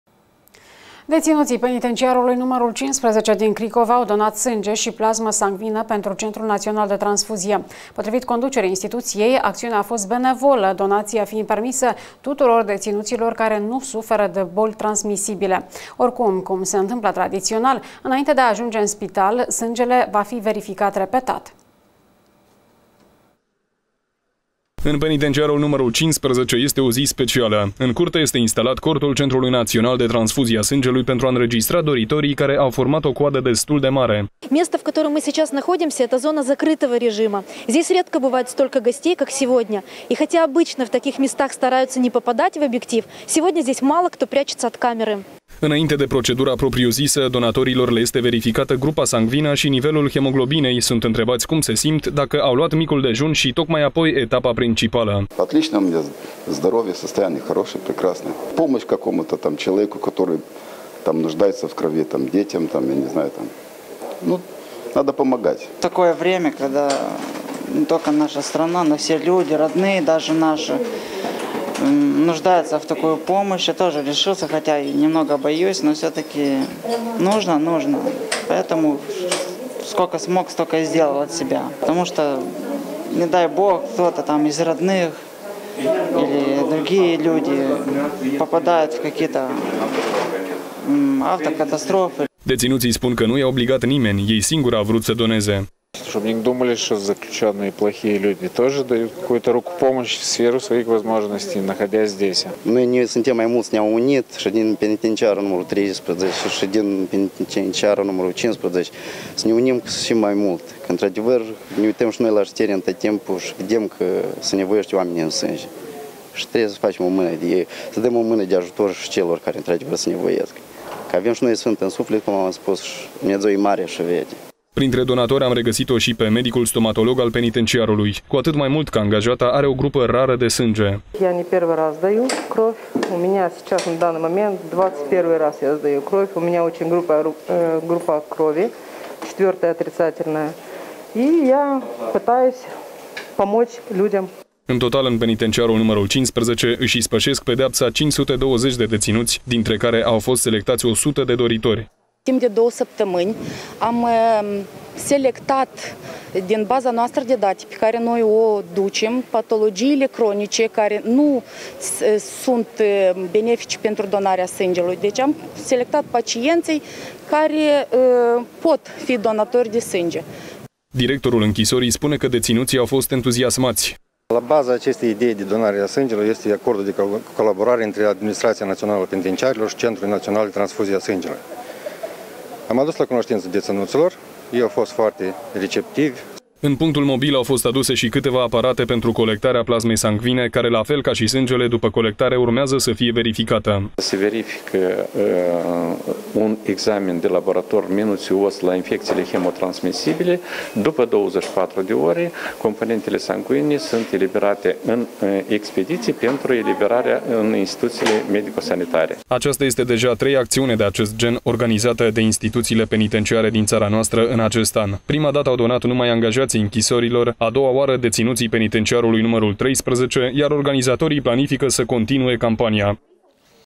AJUTOR DE LA DEȚINUȚI
Deținuții penitenciarului nr. 15 din Cricova au donat sânge și plasmă sangvină pentru Centrul Național de Transfuzie. Potrivit conducerii instituției, acțiunea a fost benevolă, donația fiind permisă tuturor deținuților care nu suferă de boli transmisibile. Oricum, cum se întâmplă tradițional, înainte de a ajunge în spitale sângele va fi verificat repetat.